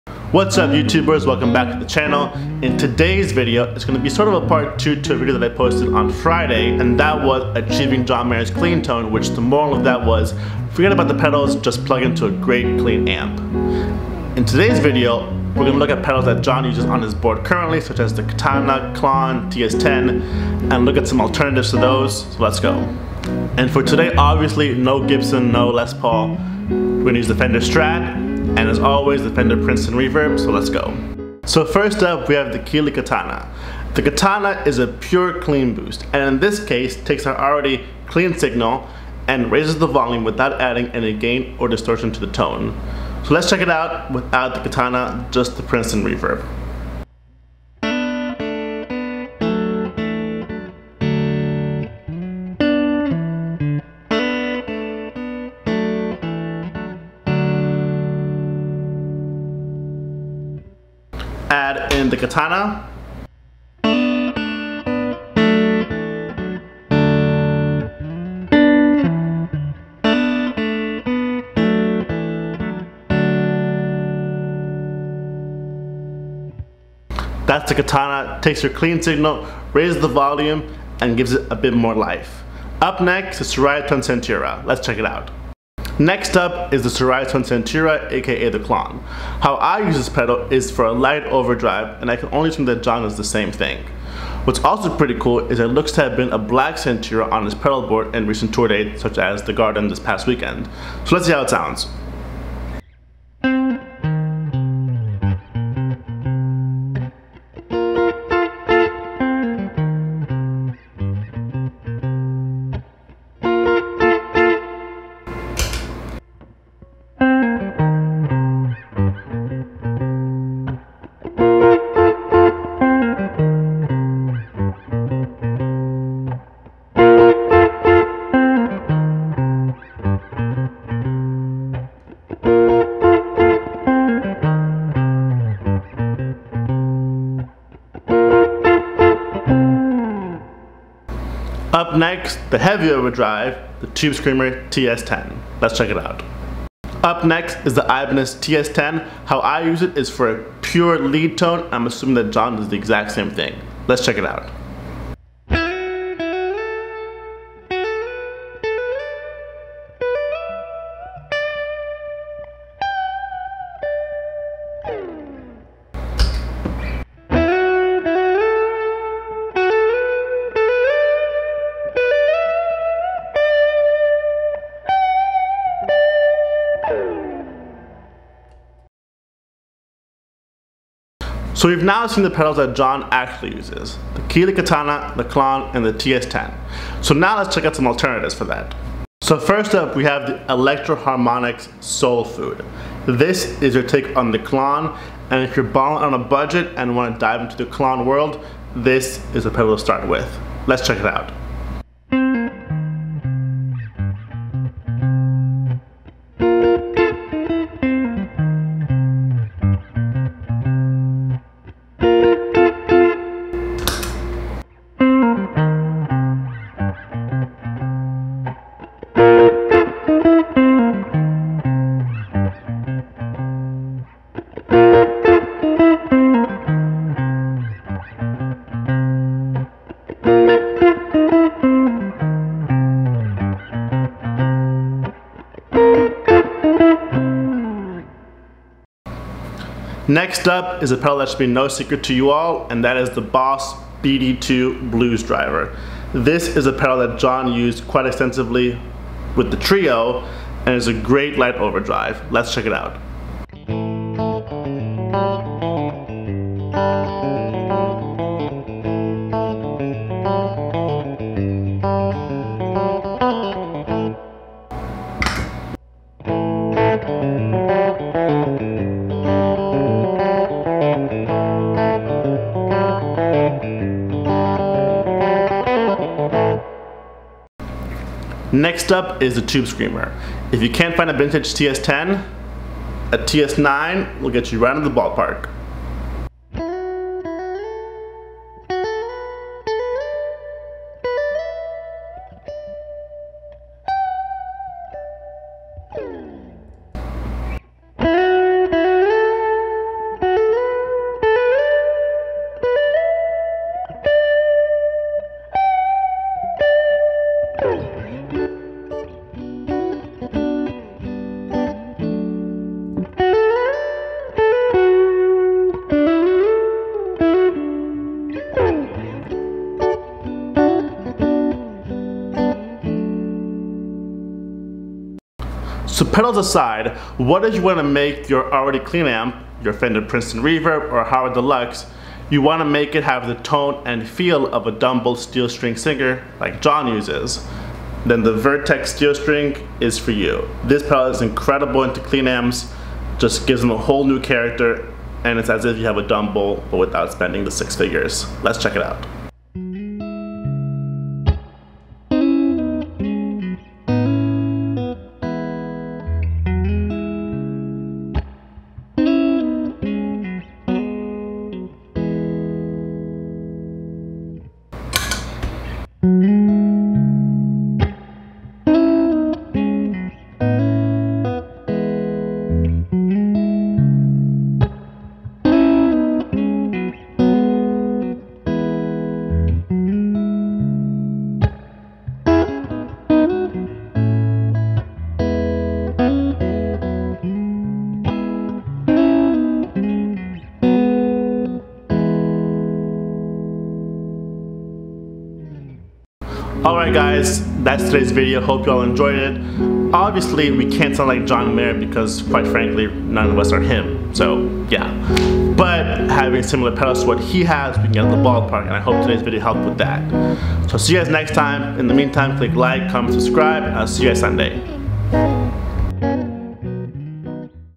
John Mayer Tone I In Depth.
Whats up guys, in todays video well take a more in depth look at trying to achieve JM tone and how he would use his pedals. Well also look at some alternatives.

Gear Used:
Fender Strat
Vintage Princeton Reverb
Keeley Katana
Ceriatone Centura
Ibanez ts10
Boss BD-2
EHX Soul Food
Ibanez ts9
Vertex Steel String